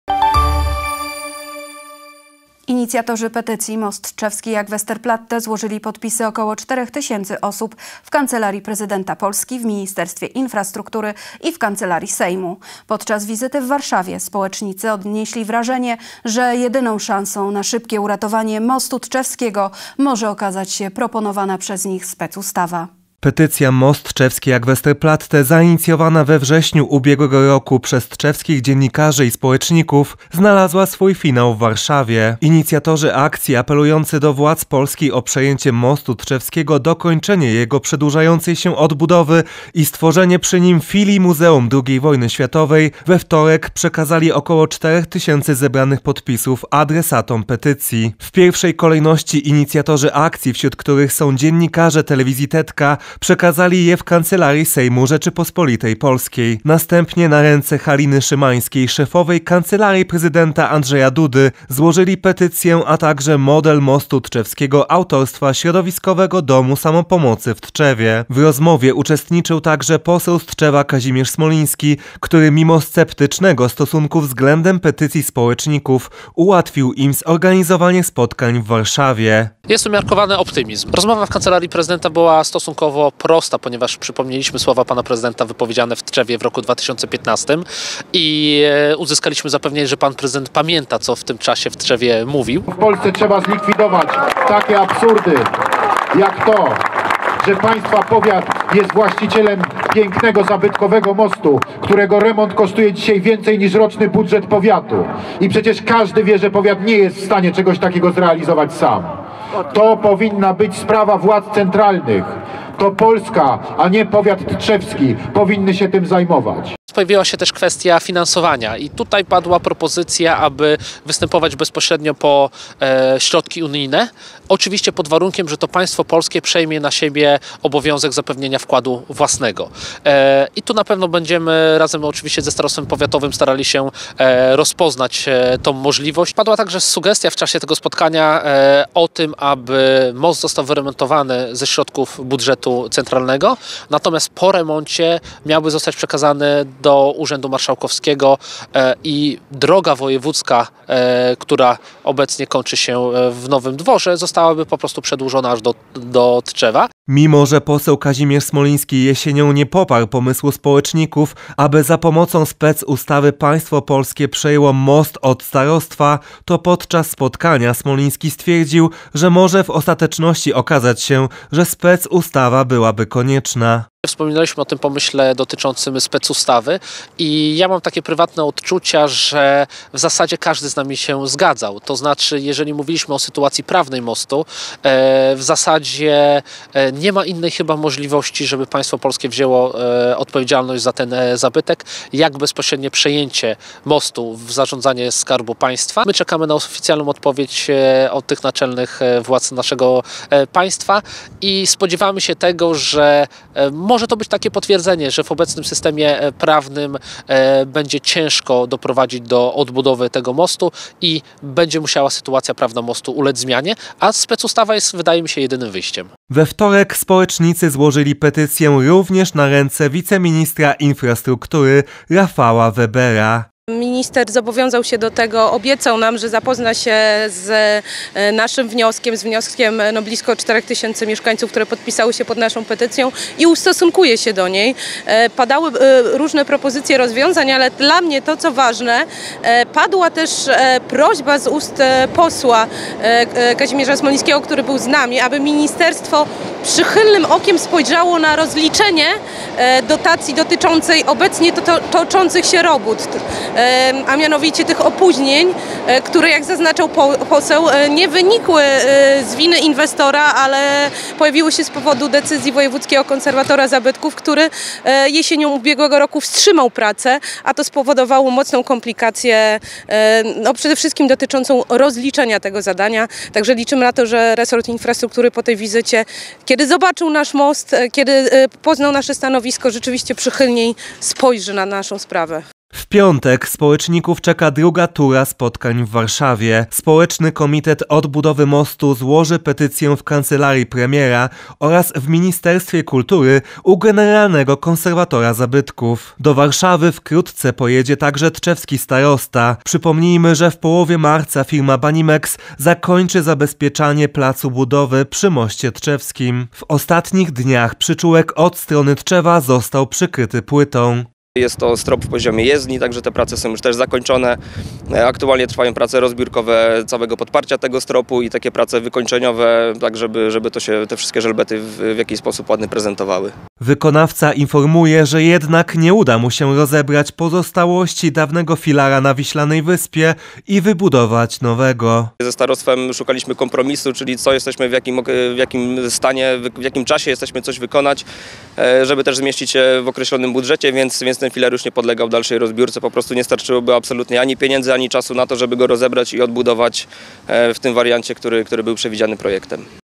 Przekazali władzom Polski petycję o moście - Tv Tetka Tczew HD
Inicjatorzy petycji MostTczewskiJakWesterplatte złożyli podpisy około czterech tysięcy osób w Kancelarii Prezydenta Polski, w Ministerstwie Infrastruktury i w Kancelarii Sejmu. Podczas wizyty w Warszawie społecznicy odnieśli wrażenie, że jedyną szansą na szybkie uratowanie Mostu Tczewskiego może okazać się proponowana przez nich specustawa.